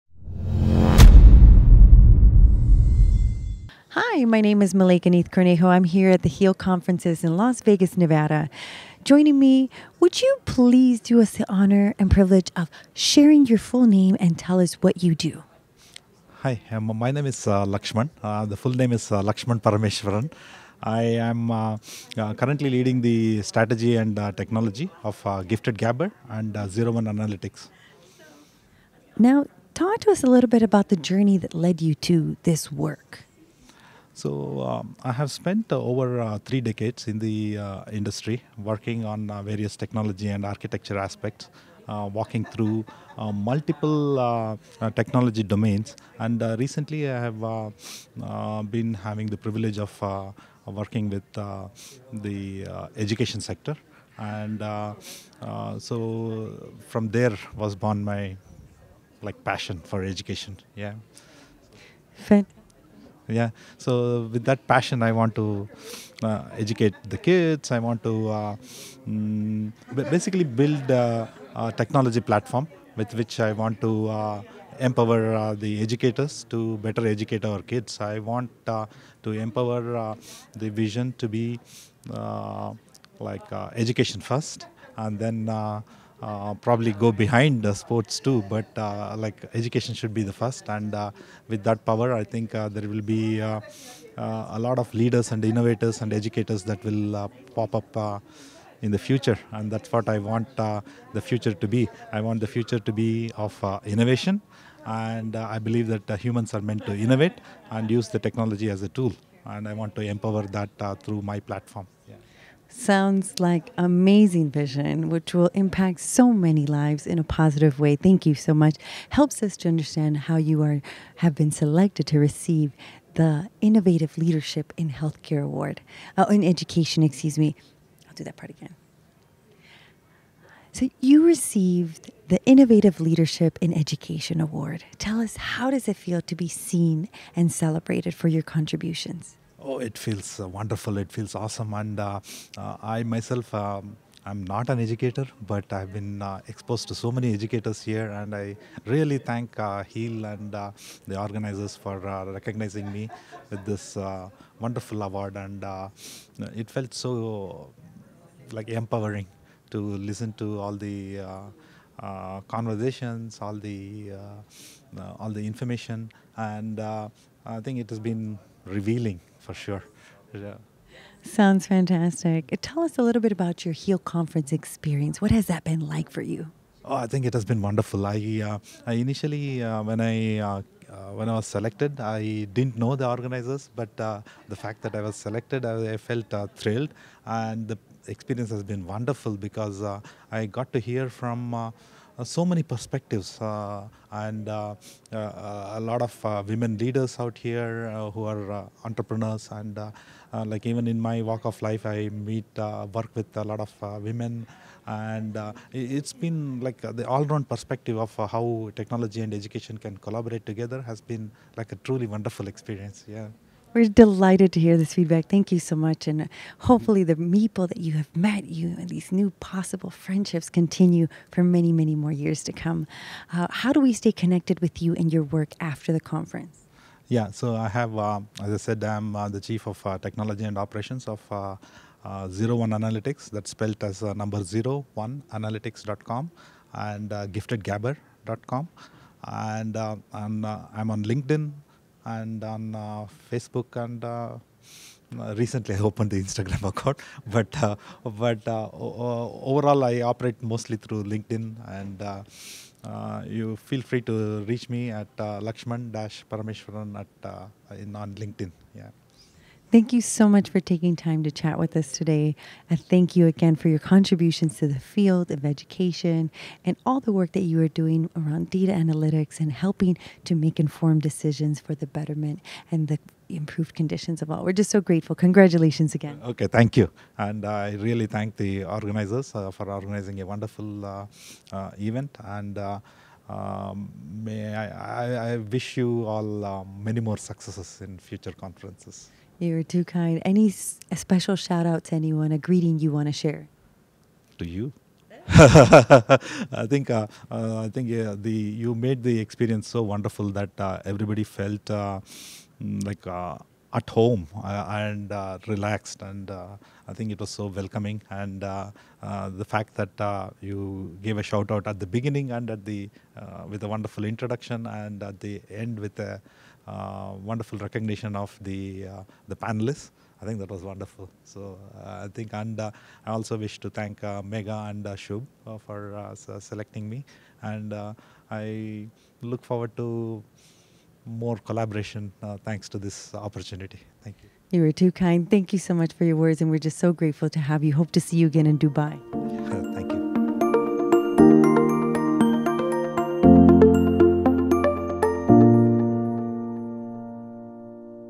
Review from Lakshman Parameswaran | HEaL Conferences 2024
Lakshman Parameswaran, CTO at Zero One Analytics, Inc. and Gifted Gabber, Inc. shared his journey throughout the three incredible days of HEaL Conferences. The prestigious event took place at the Golden Nugget in Las Vegas, USA, from June 14th to 16th, 2024.